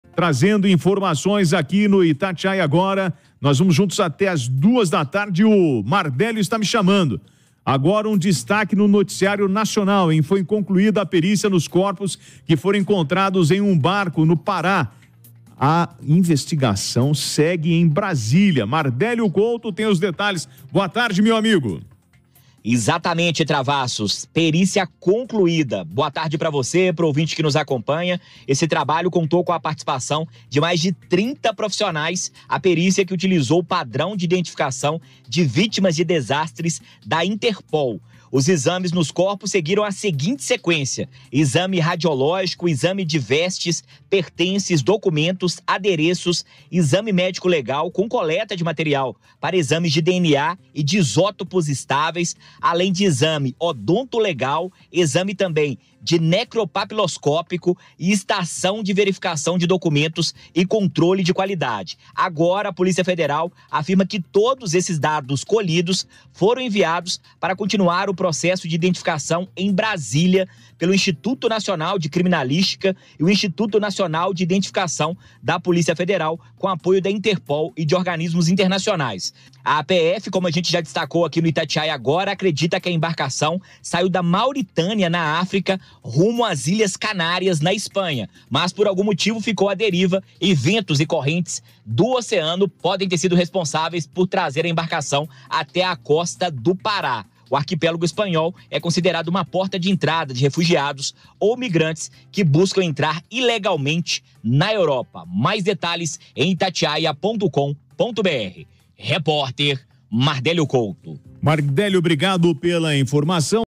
PF UTILIZOU O PROTOCOLO DE IDENTIFICAÇÃO DE VÍTIMAS DE DESASTRES DA INTERPOL PARA PERICIAR CORPOS
A Polícia Federal e a Polícia Científica do Pará concluíram a perícia nos noves corpos encontrados no barco que foi localizado à deriva próximo a Bragança, na costa do paraense.